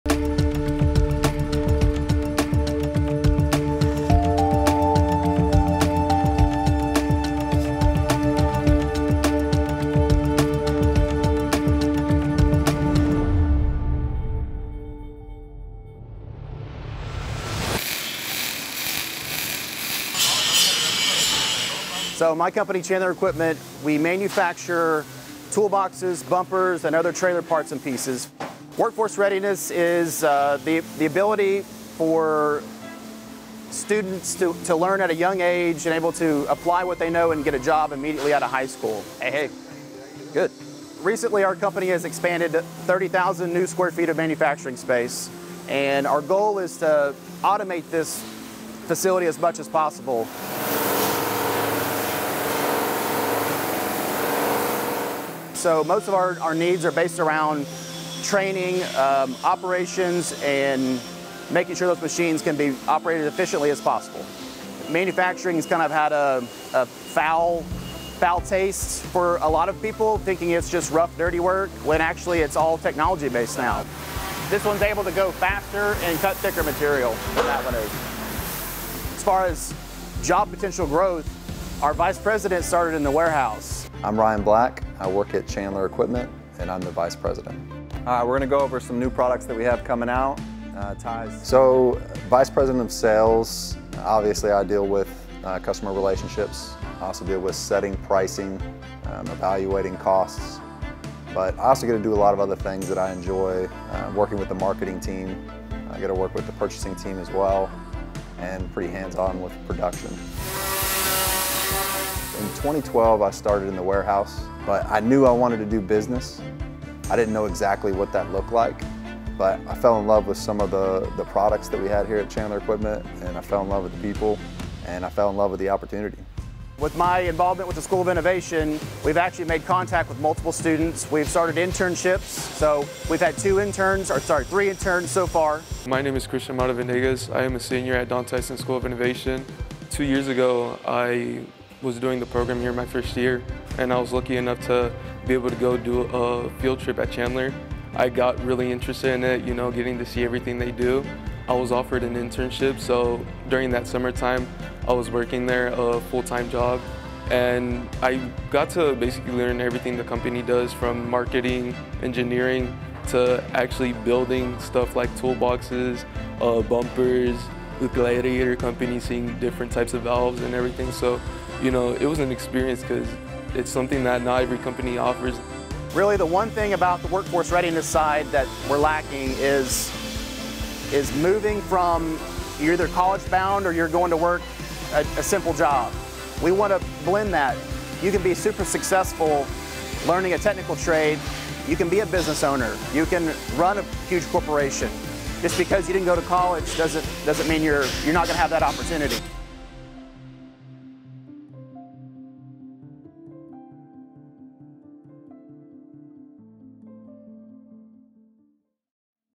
Workforce Readiness | Chandler Equipment
The Springdale Public Schools Workforce Readiness video series features talent pathway partnerships with local industry. This video features Chandler Equipment in Springdale and how the company offers careers for Manufacturing, Robotics maintenance & operation, Sales and other positions with in the company.